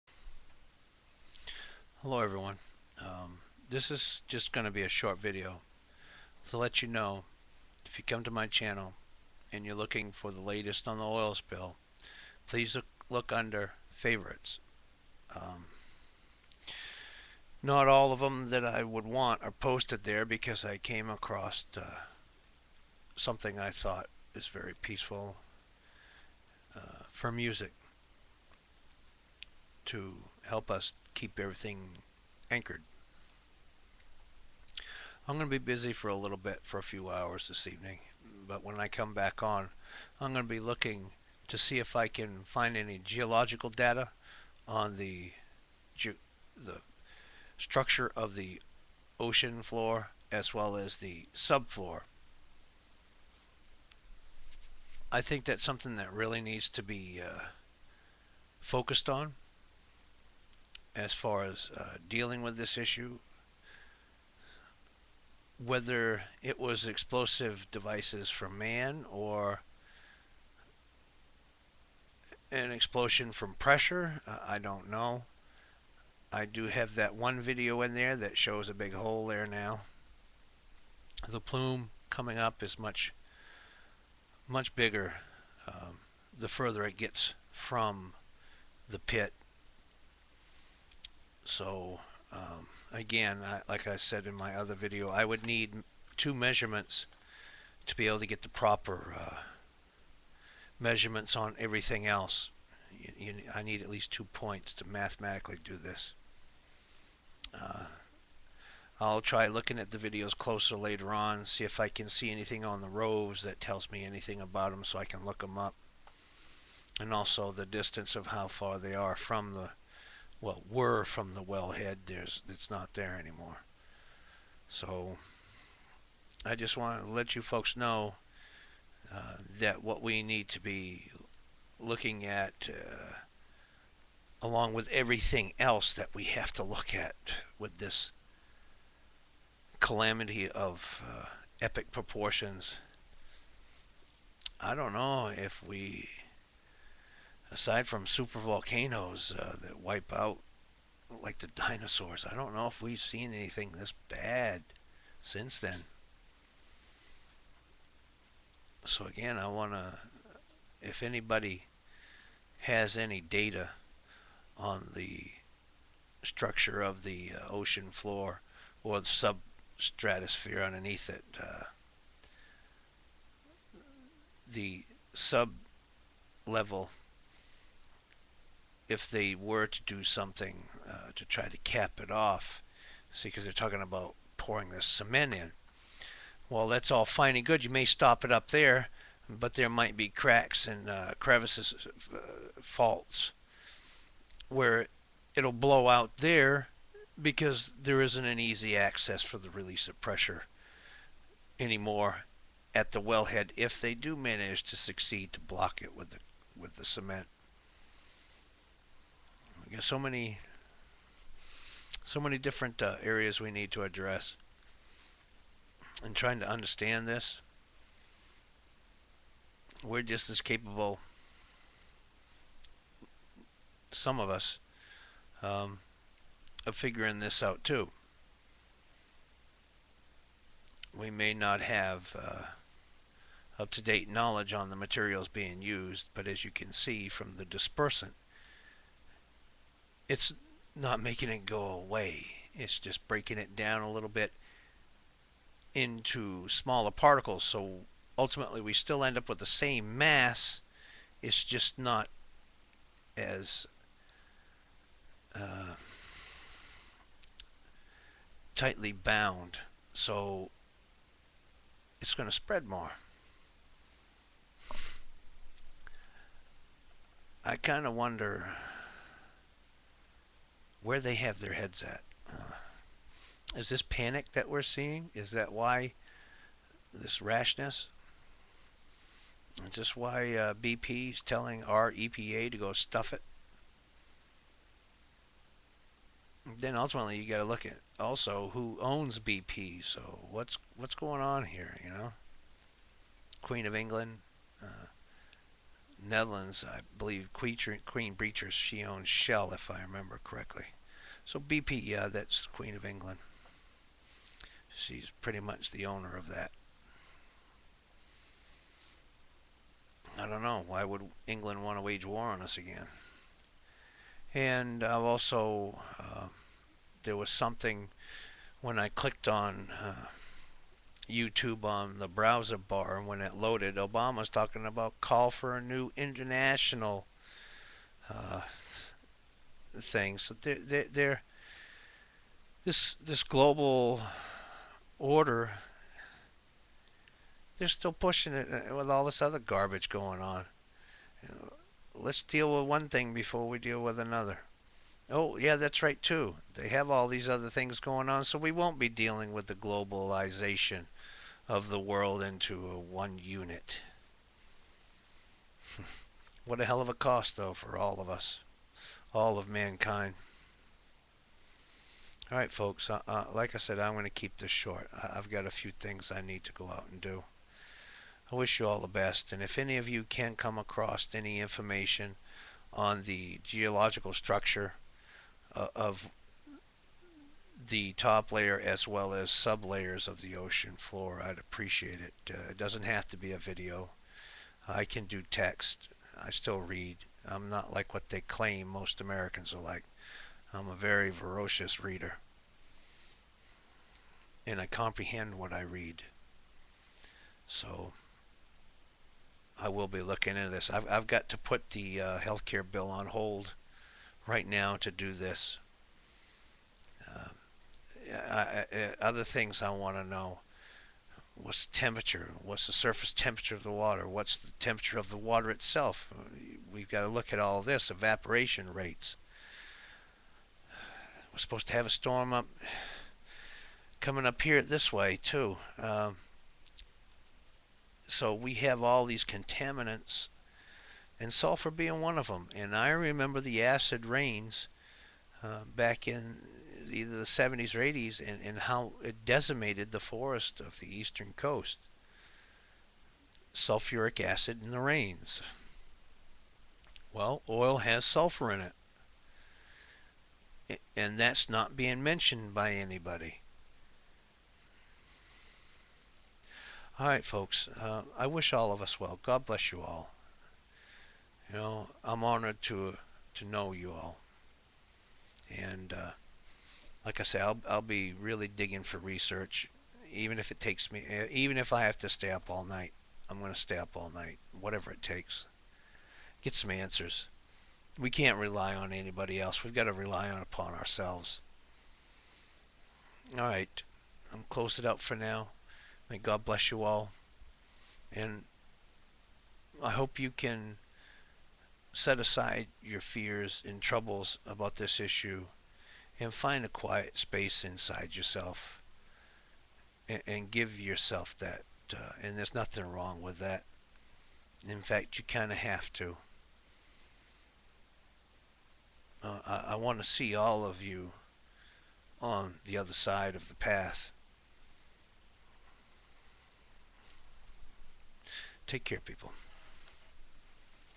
SKeene1956 as NotLuckyInLife: Oil Spill; Floor Struture? Evaporation Rates? Temps?
NotLuckyInLife's webcam video May 25, 2010, 03:20 PM; A lot of questions and no answers. I will go all night after I run my errands if need be to try to find this data and extrapulate it to a format that can be understood. I wish all of you well and may God be with you all.